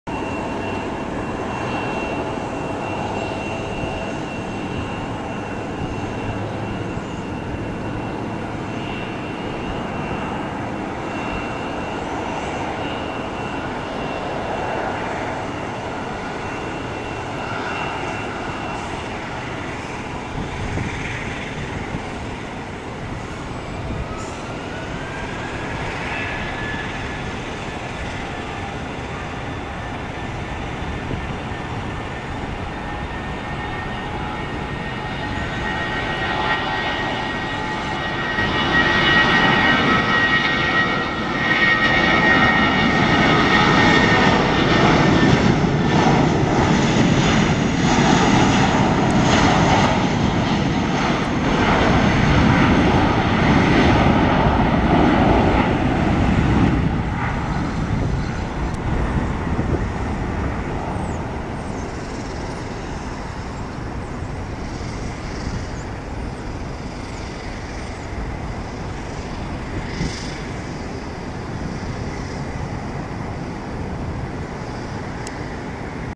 JetBlue Airways Airbus A320-232 Sharklets [N807JB] takeoff from PDX
Flight info:
Airline: JetBlue Airways
Aircraft: Airbus A320-232(WL)
Reg: N807JB
Airport: Portland International Airport
Flight #: B6 1621
Route: PDX-LGB
Runway: 28L
Date: 7/7/13
Time: 3:45PM
Comments: "1. Fly JetBlue 2. Repeat step 1"